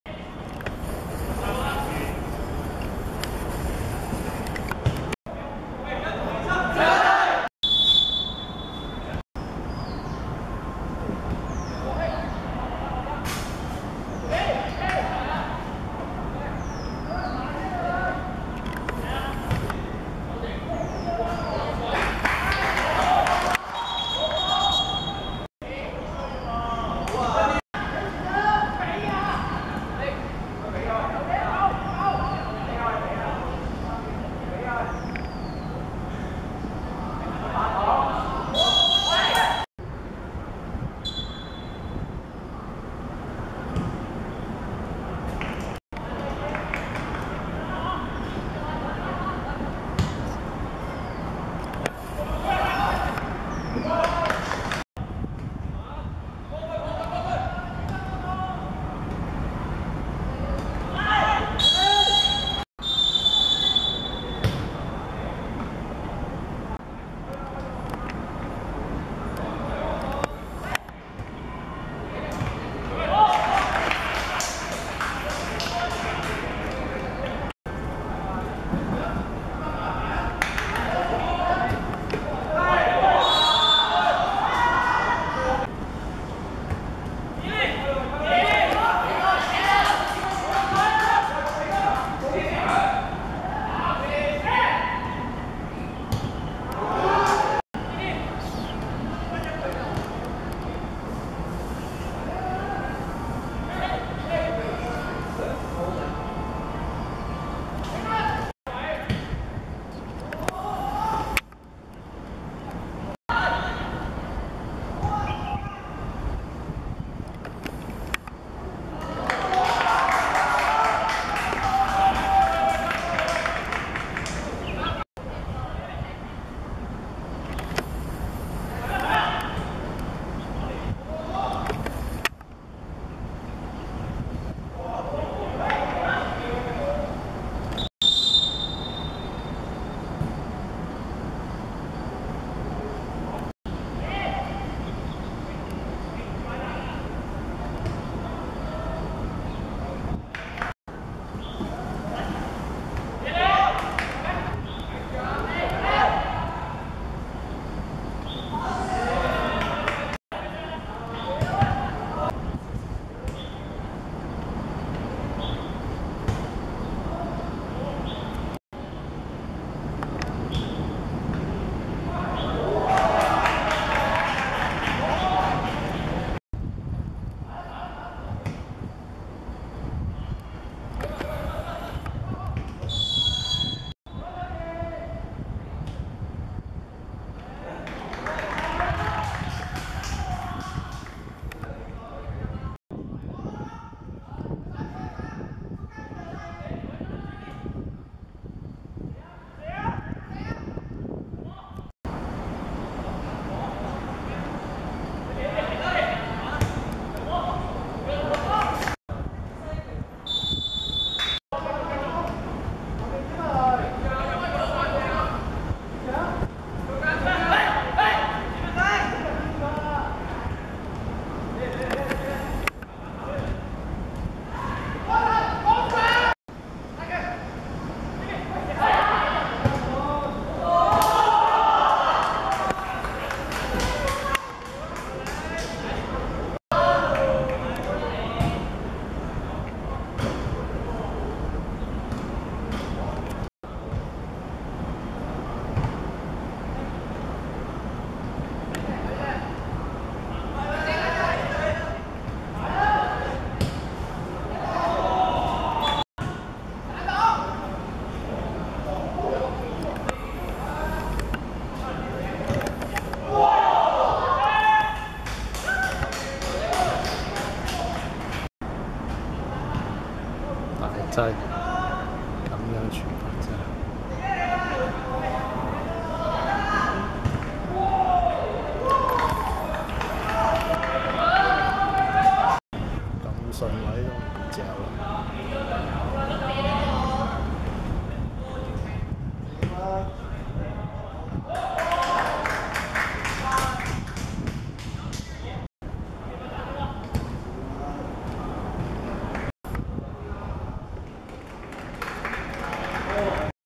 何玉清vs聖方濟(2015.11.4.D3K2學界足球甲組)精華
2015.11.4.蒲崗村仿真草塲 13:00 何玉清(紅) 0 : 8 聖方濟(白)